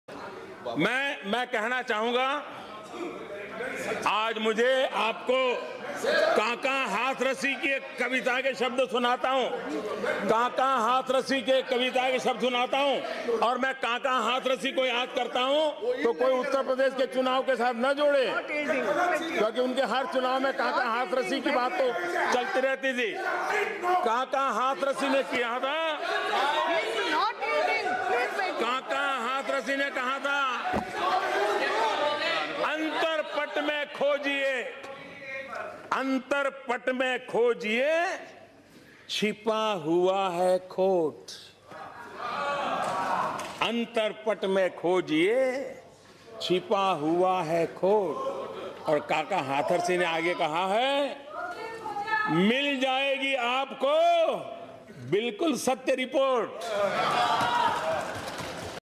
PM recites Kaka Hathrasi Quote, Antarpat mei Khojiye...Chipa hua hai khot
Prime Minister Narendra Modi recites Kaka Hathrasi Quote during his speech in Lok Sabha
अंतरपट में खोजिए, छिपा हुआ है खोट,
मिल जाएगी आपको, बिल्कुल सत्य रिपोट